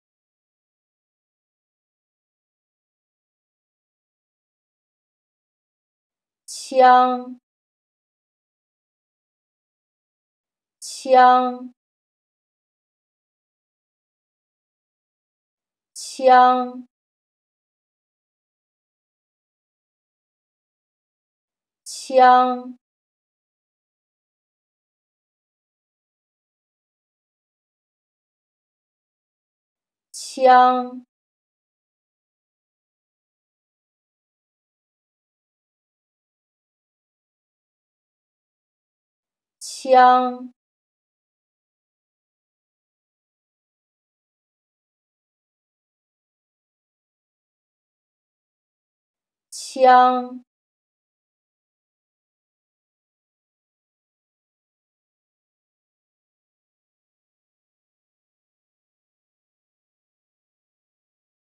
How To Say 'Gun' (枪) in Mandarin Chinese
Learn how to pronounce ''Gun'' (枪) in Mandarin Chinese with this short pronunciation video